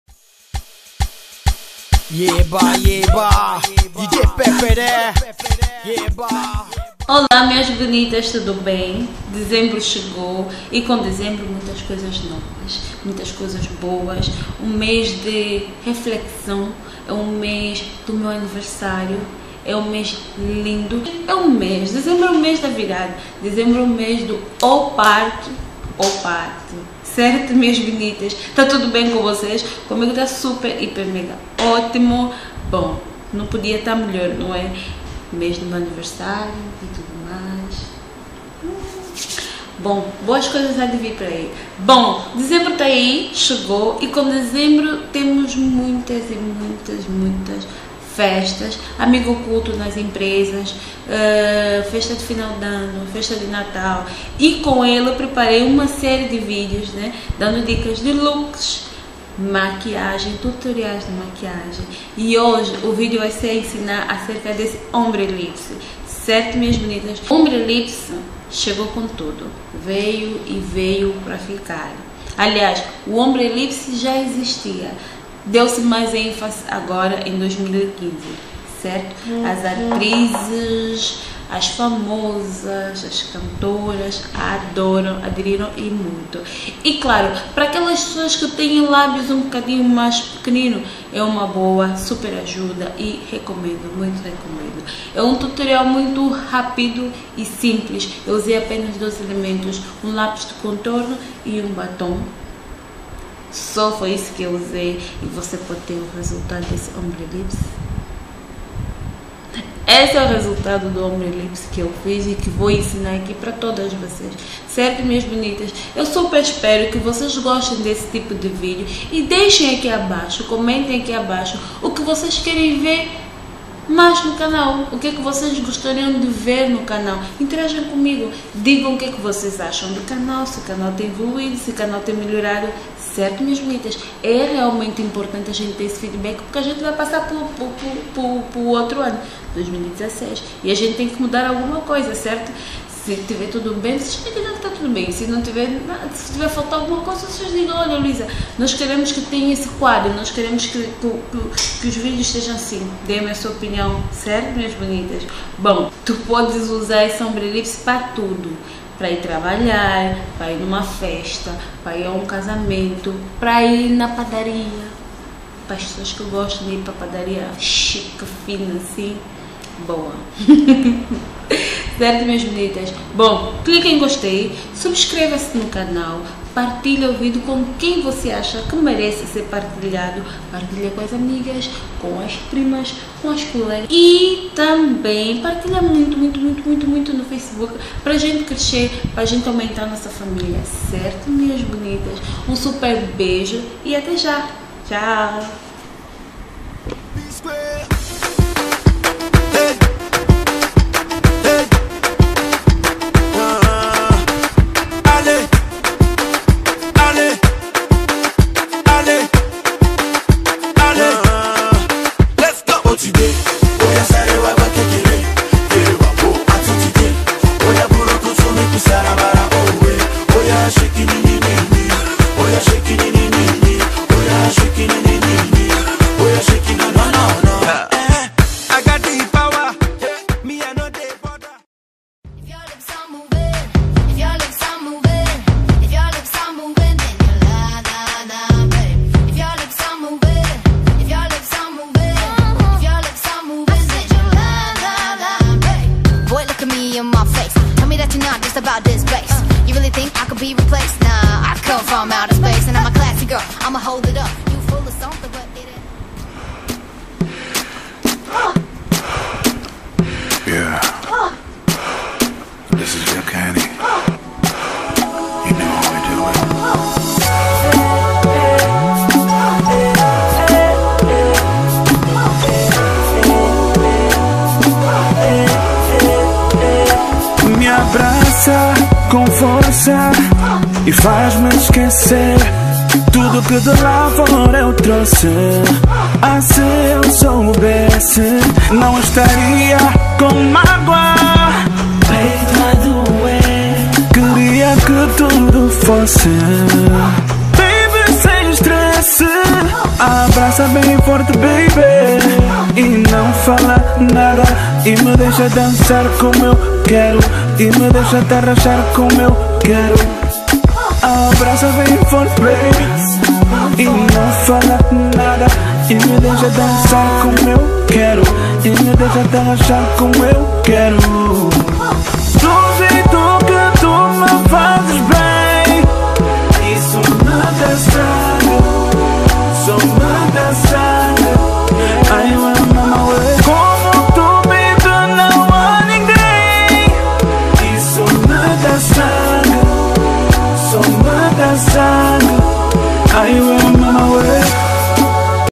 Ombre Lips 💋👄💋|Luísa Fernandes 💄💄💄
Olá minhas lindas, hoje trago um tutorial rapindinho,de como fazer um ombre lips,usando apenas dois produtinhos.
Venha com a luisa e adapta aí na sua casa e arrasa com tudo